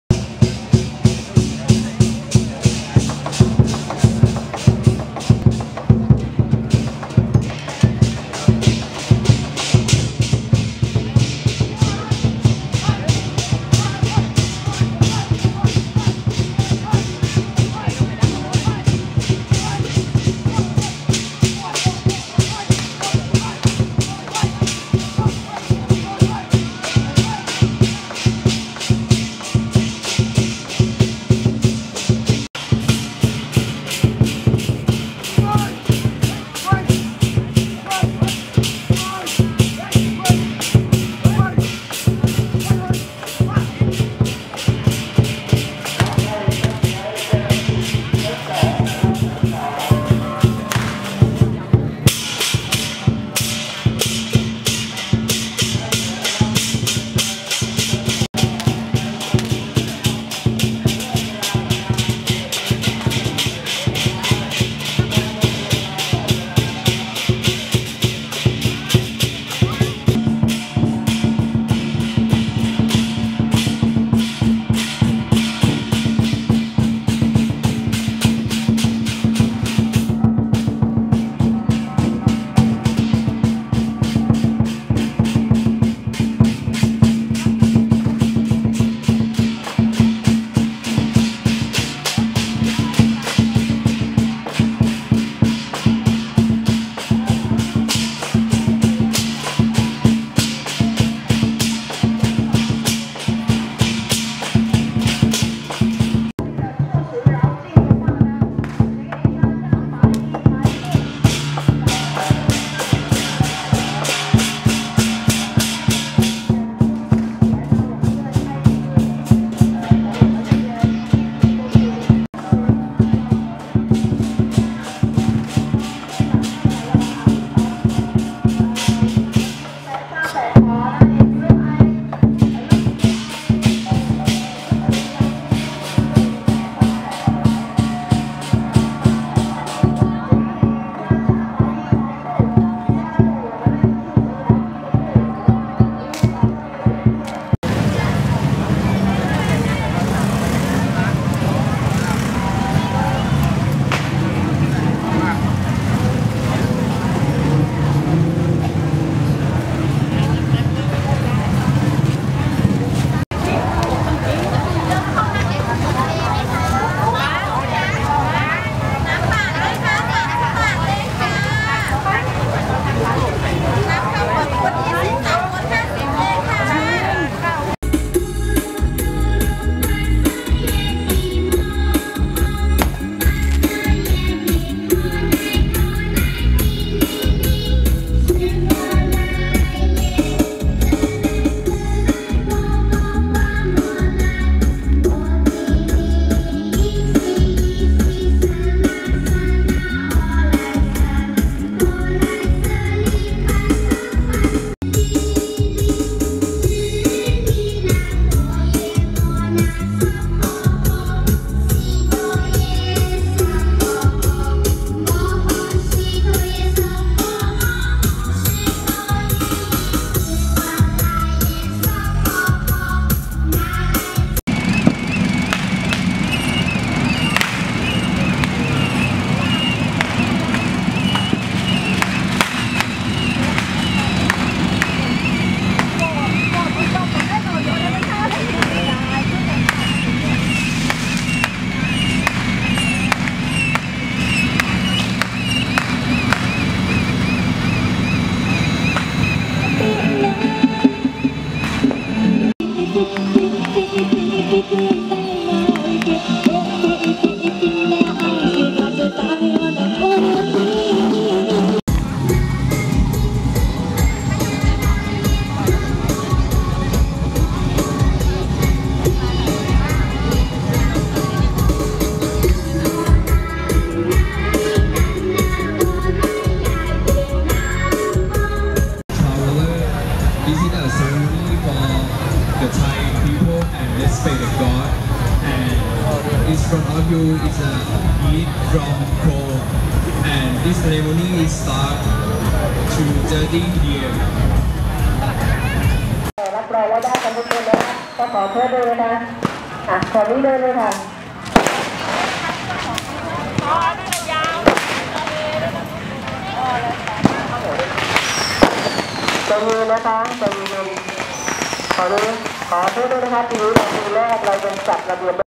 PHUKET VEGETARIAN FESTIVAL 2018, part 5 - Night Life
Night events of Phuket Vegetarian Festival

One of the most interesting and shocking festivals at Asia - Phuket Vegetarian Festival 2018.

Scarring and piercing the cheeks, climbing on the blades, pouring boiling oil and many many more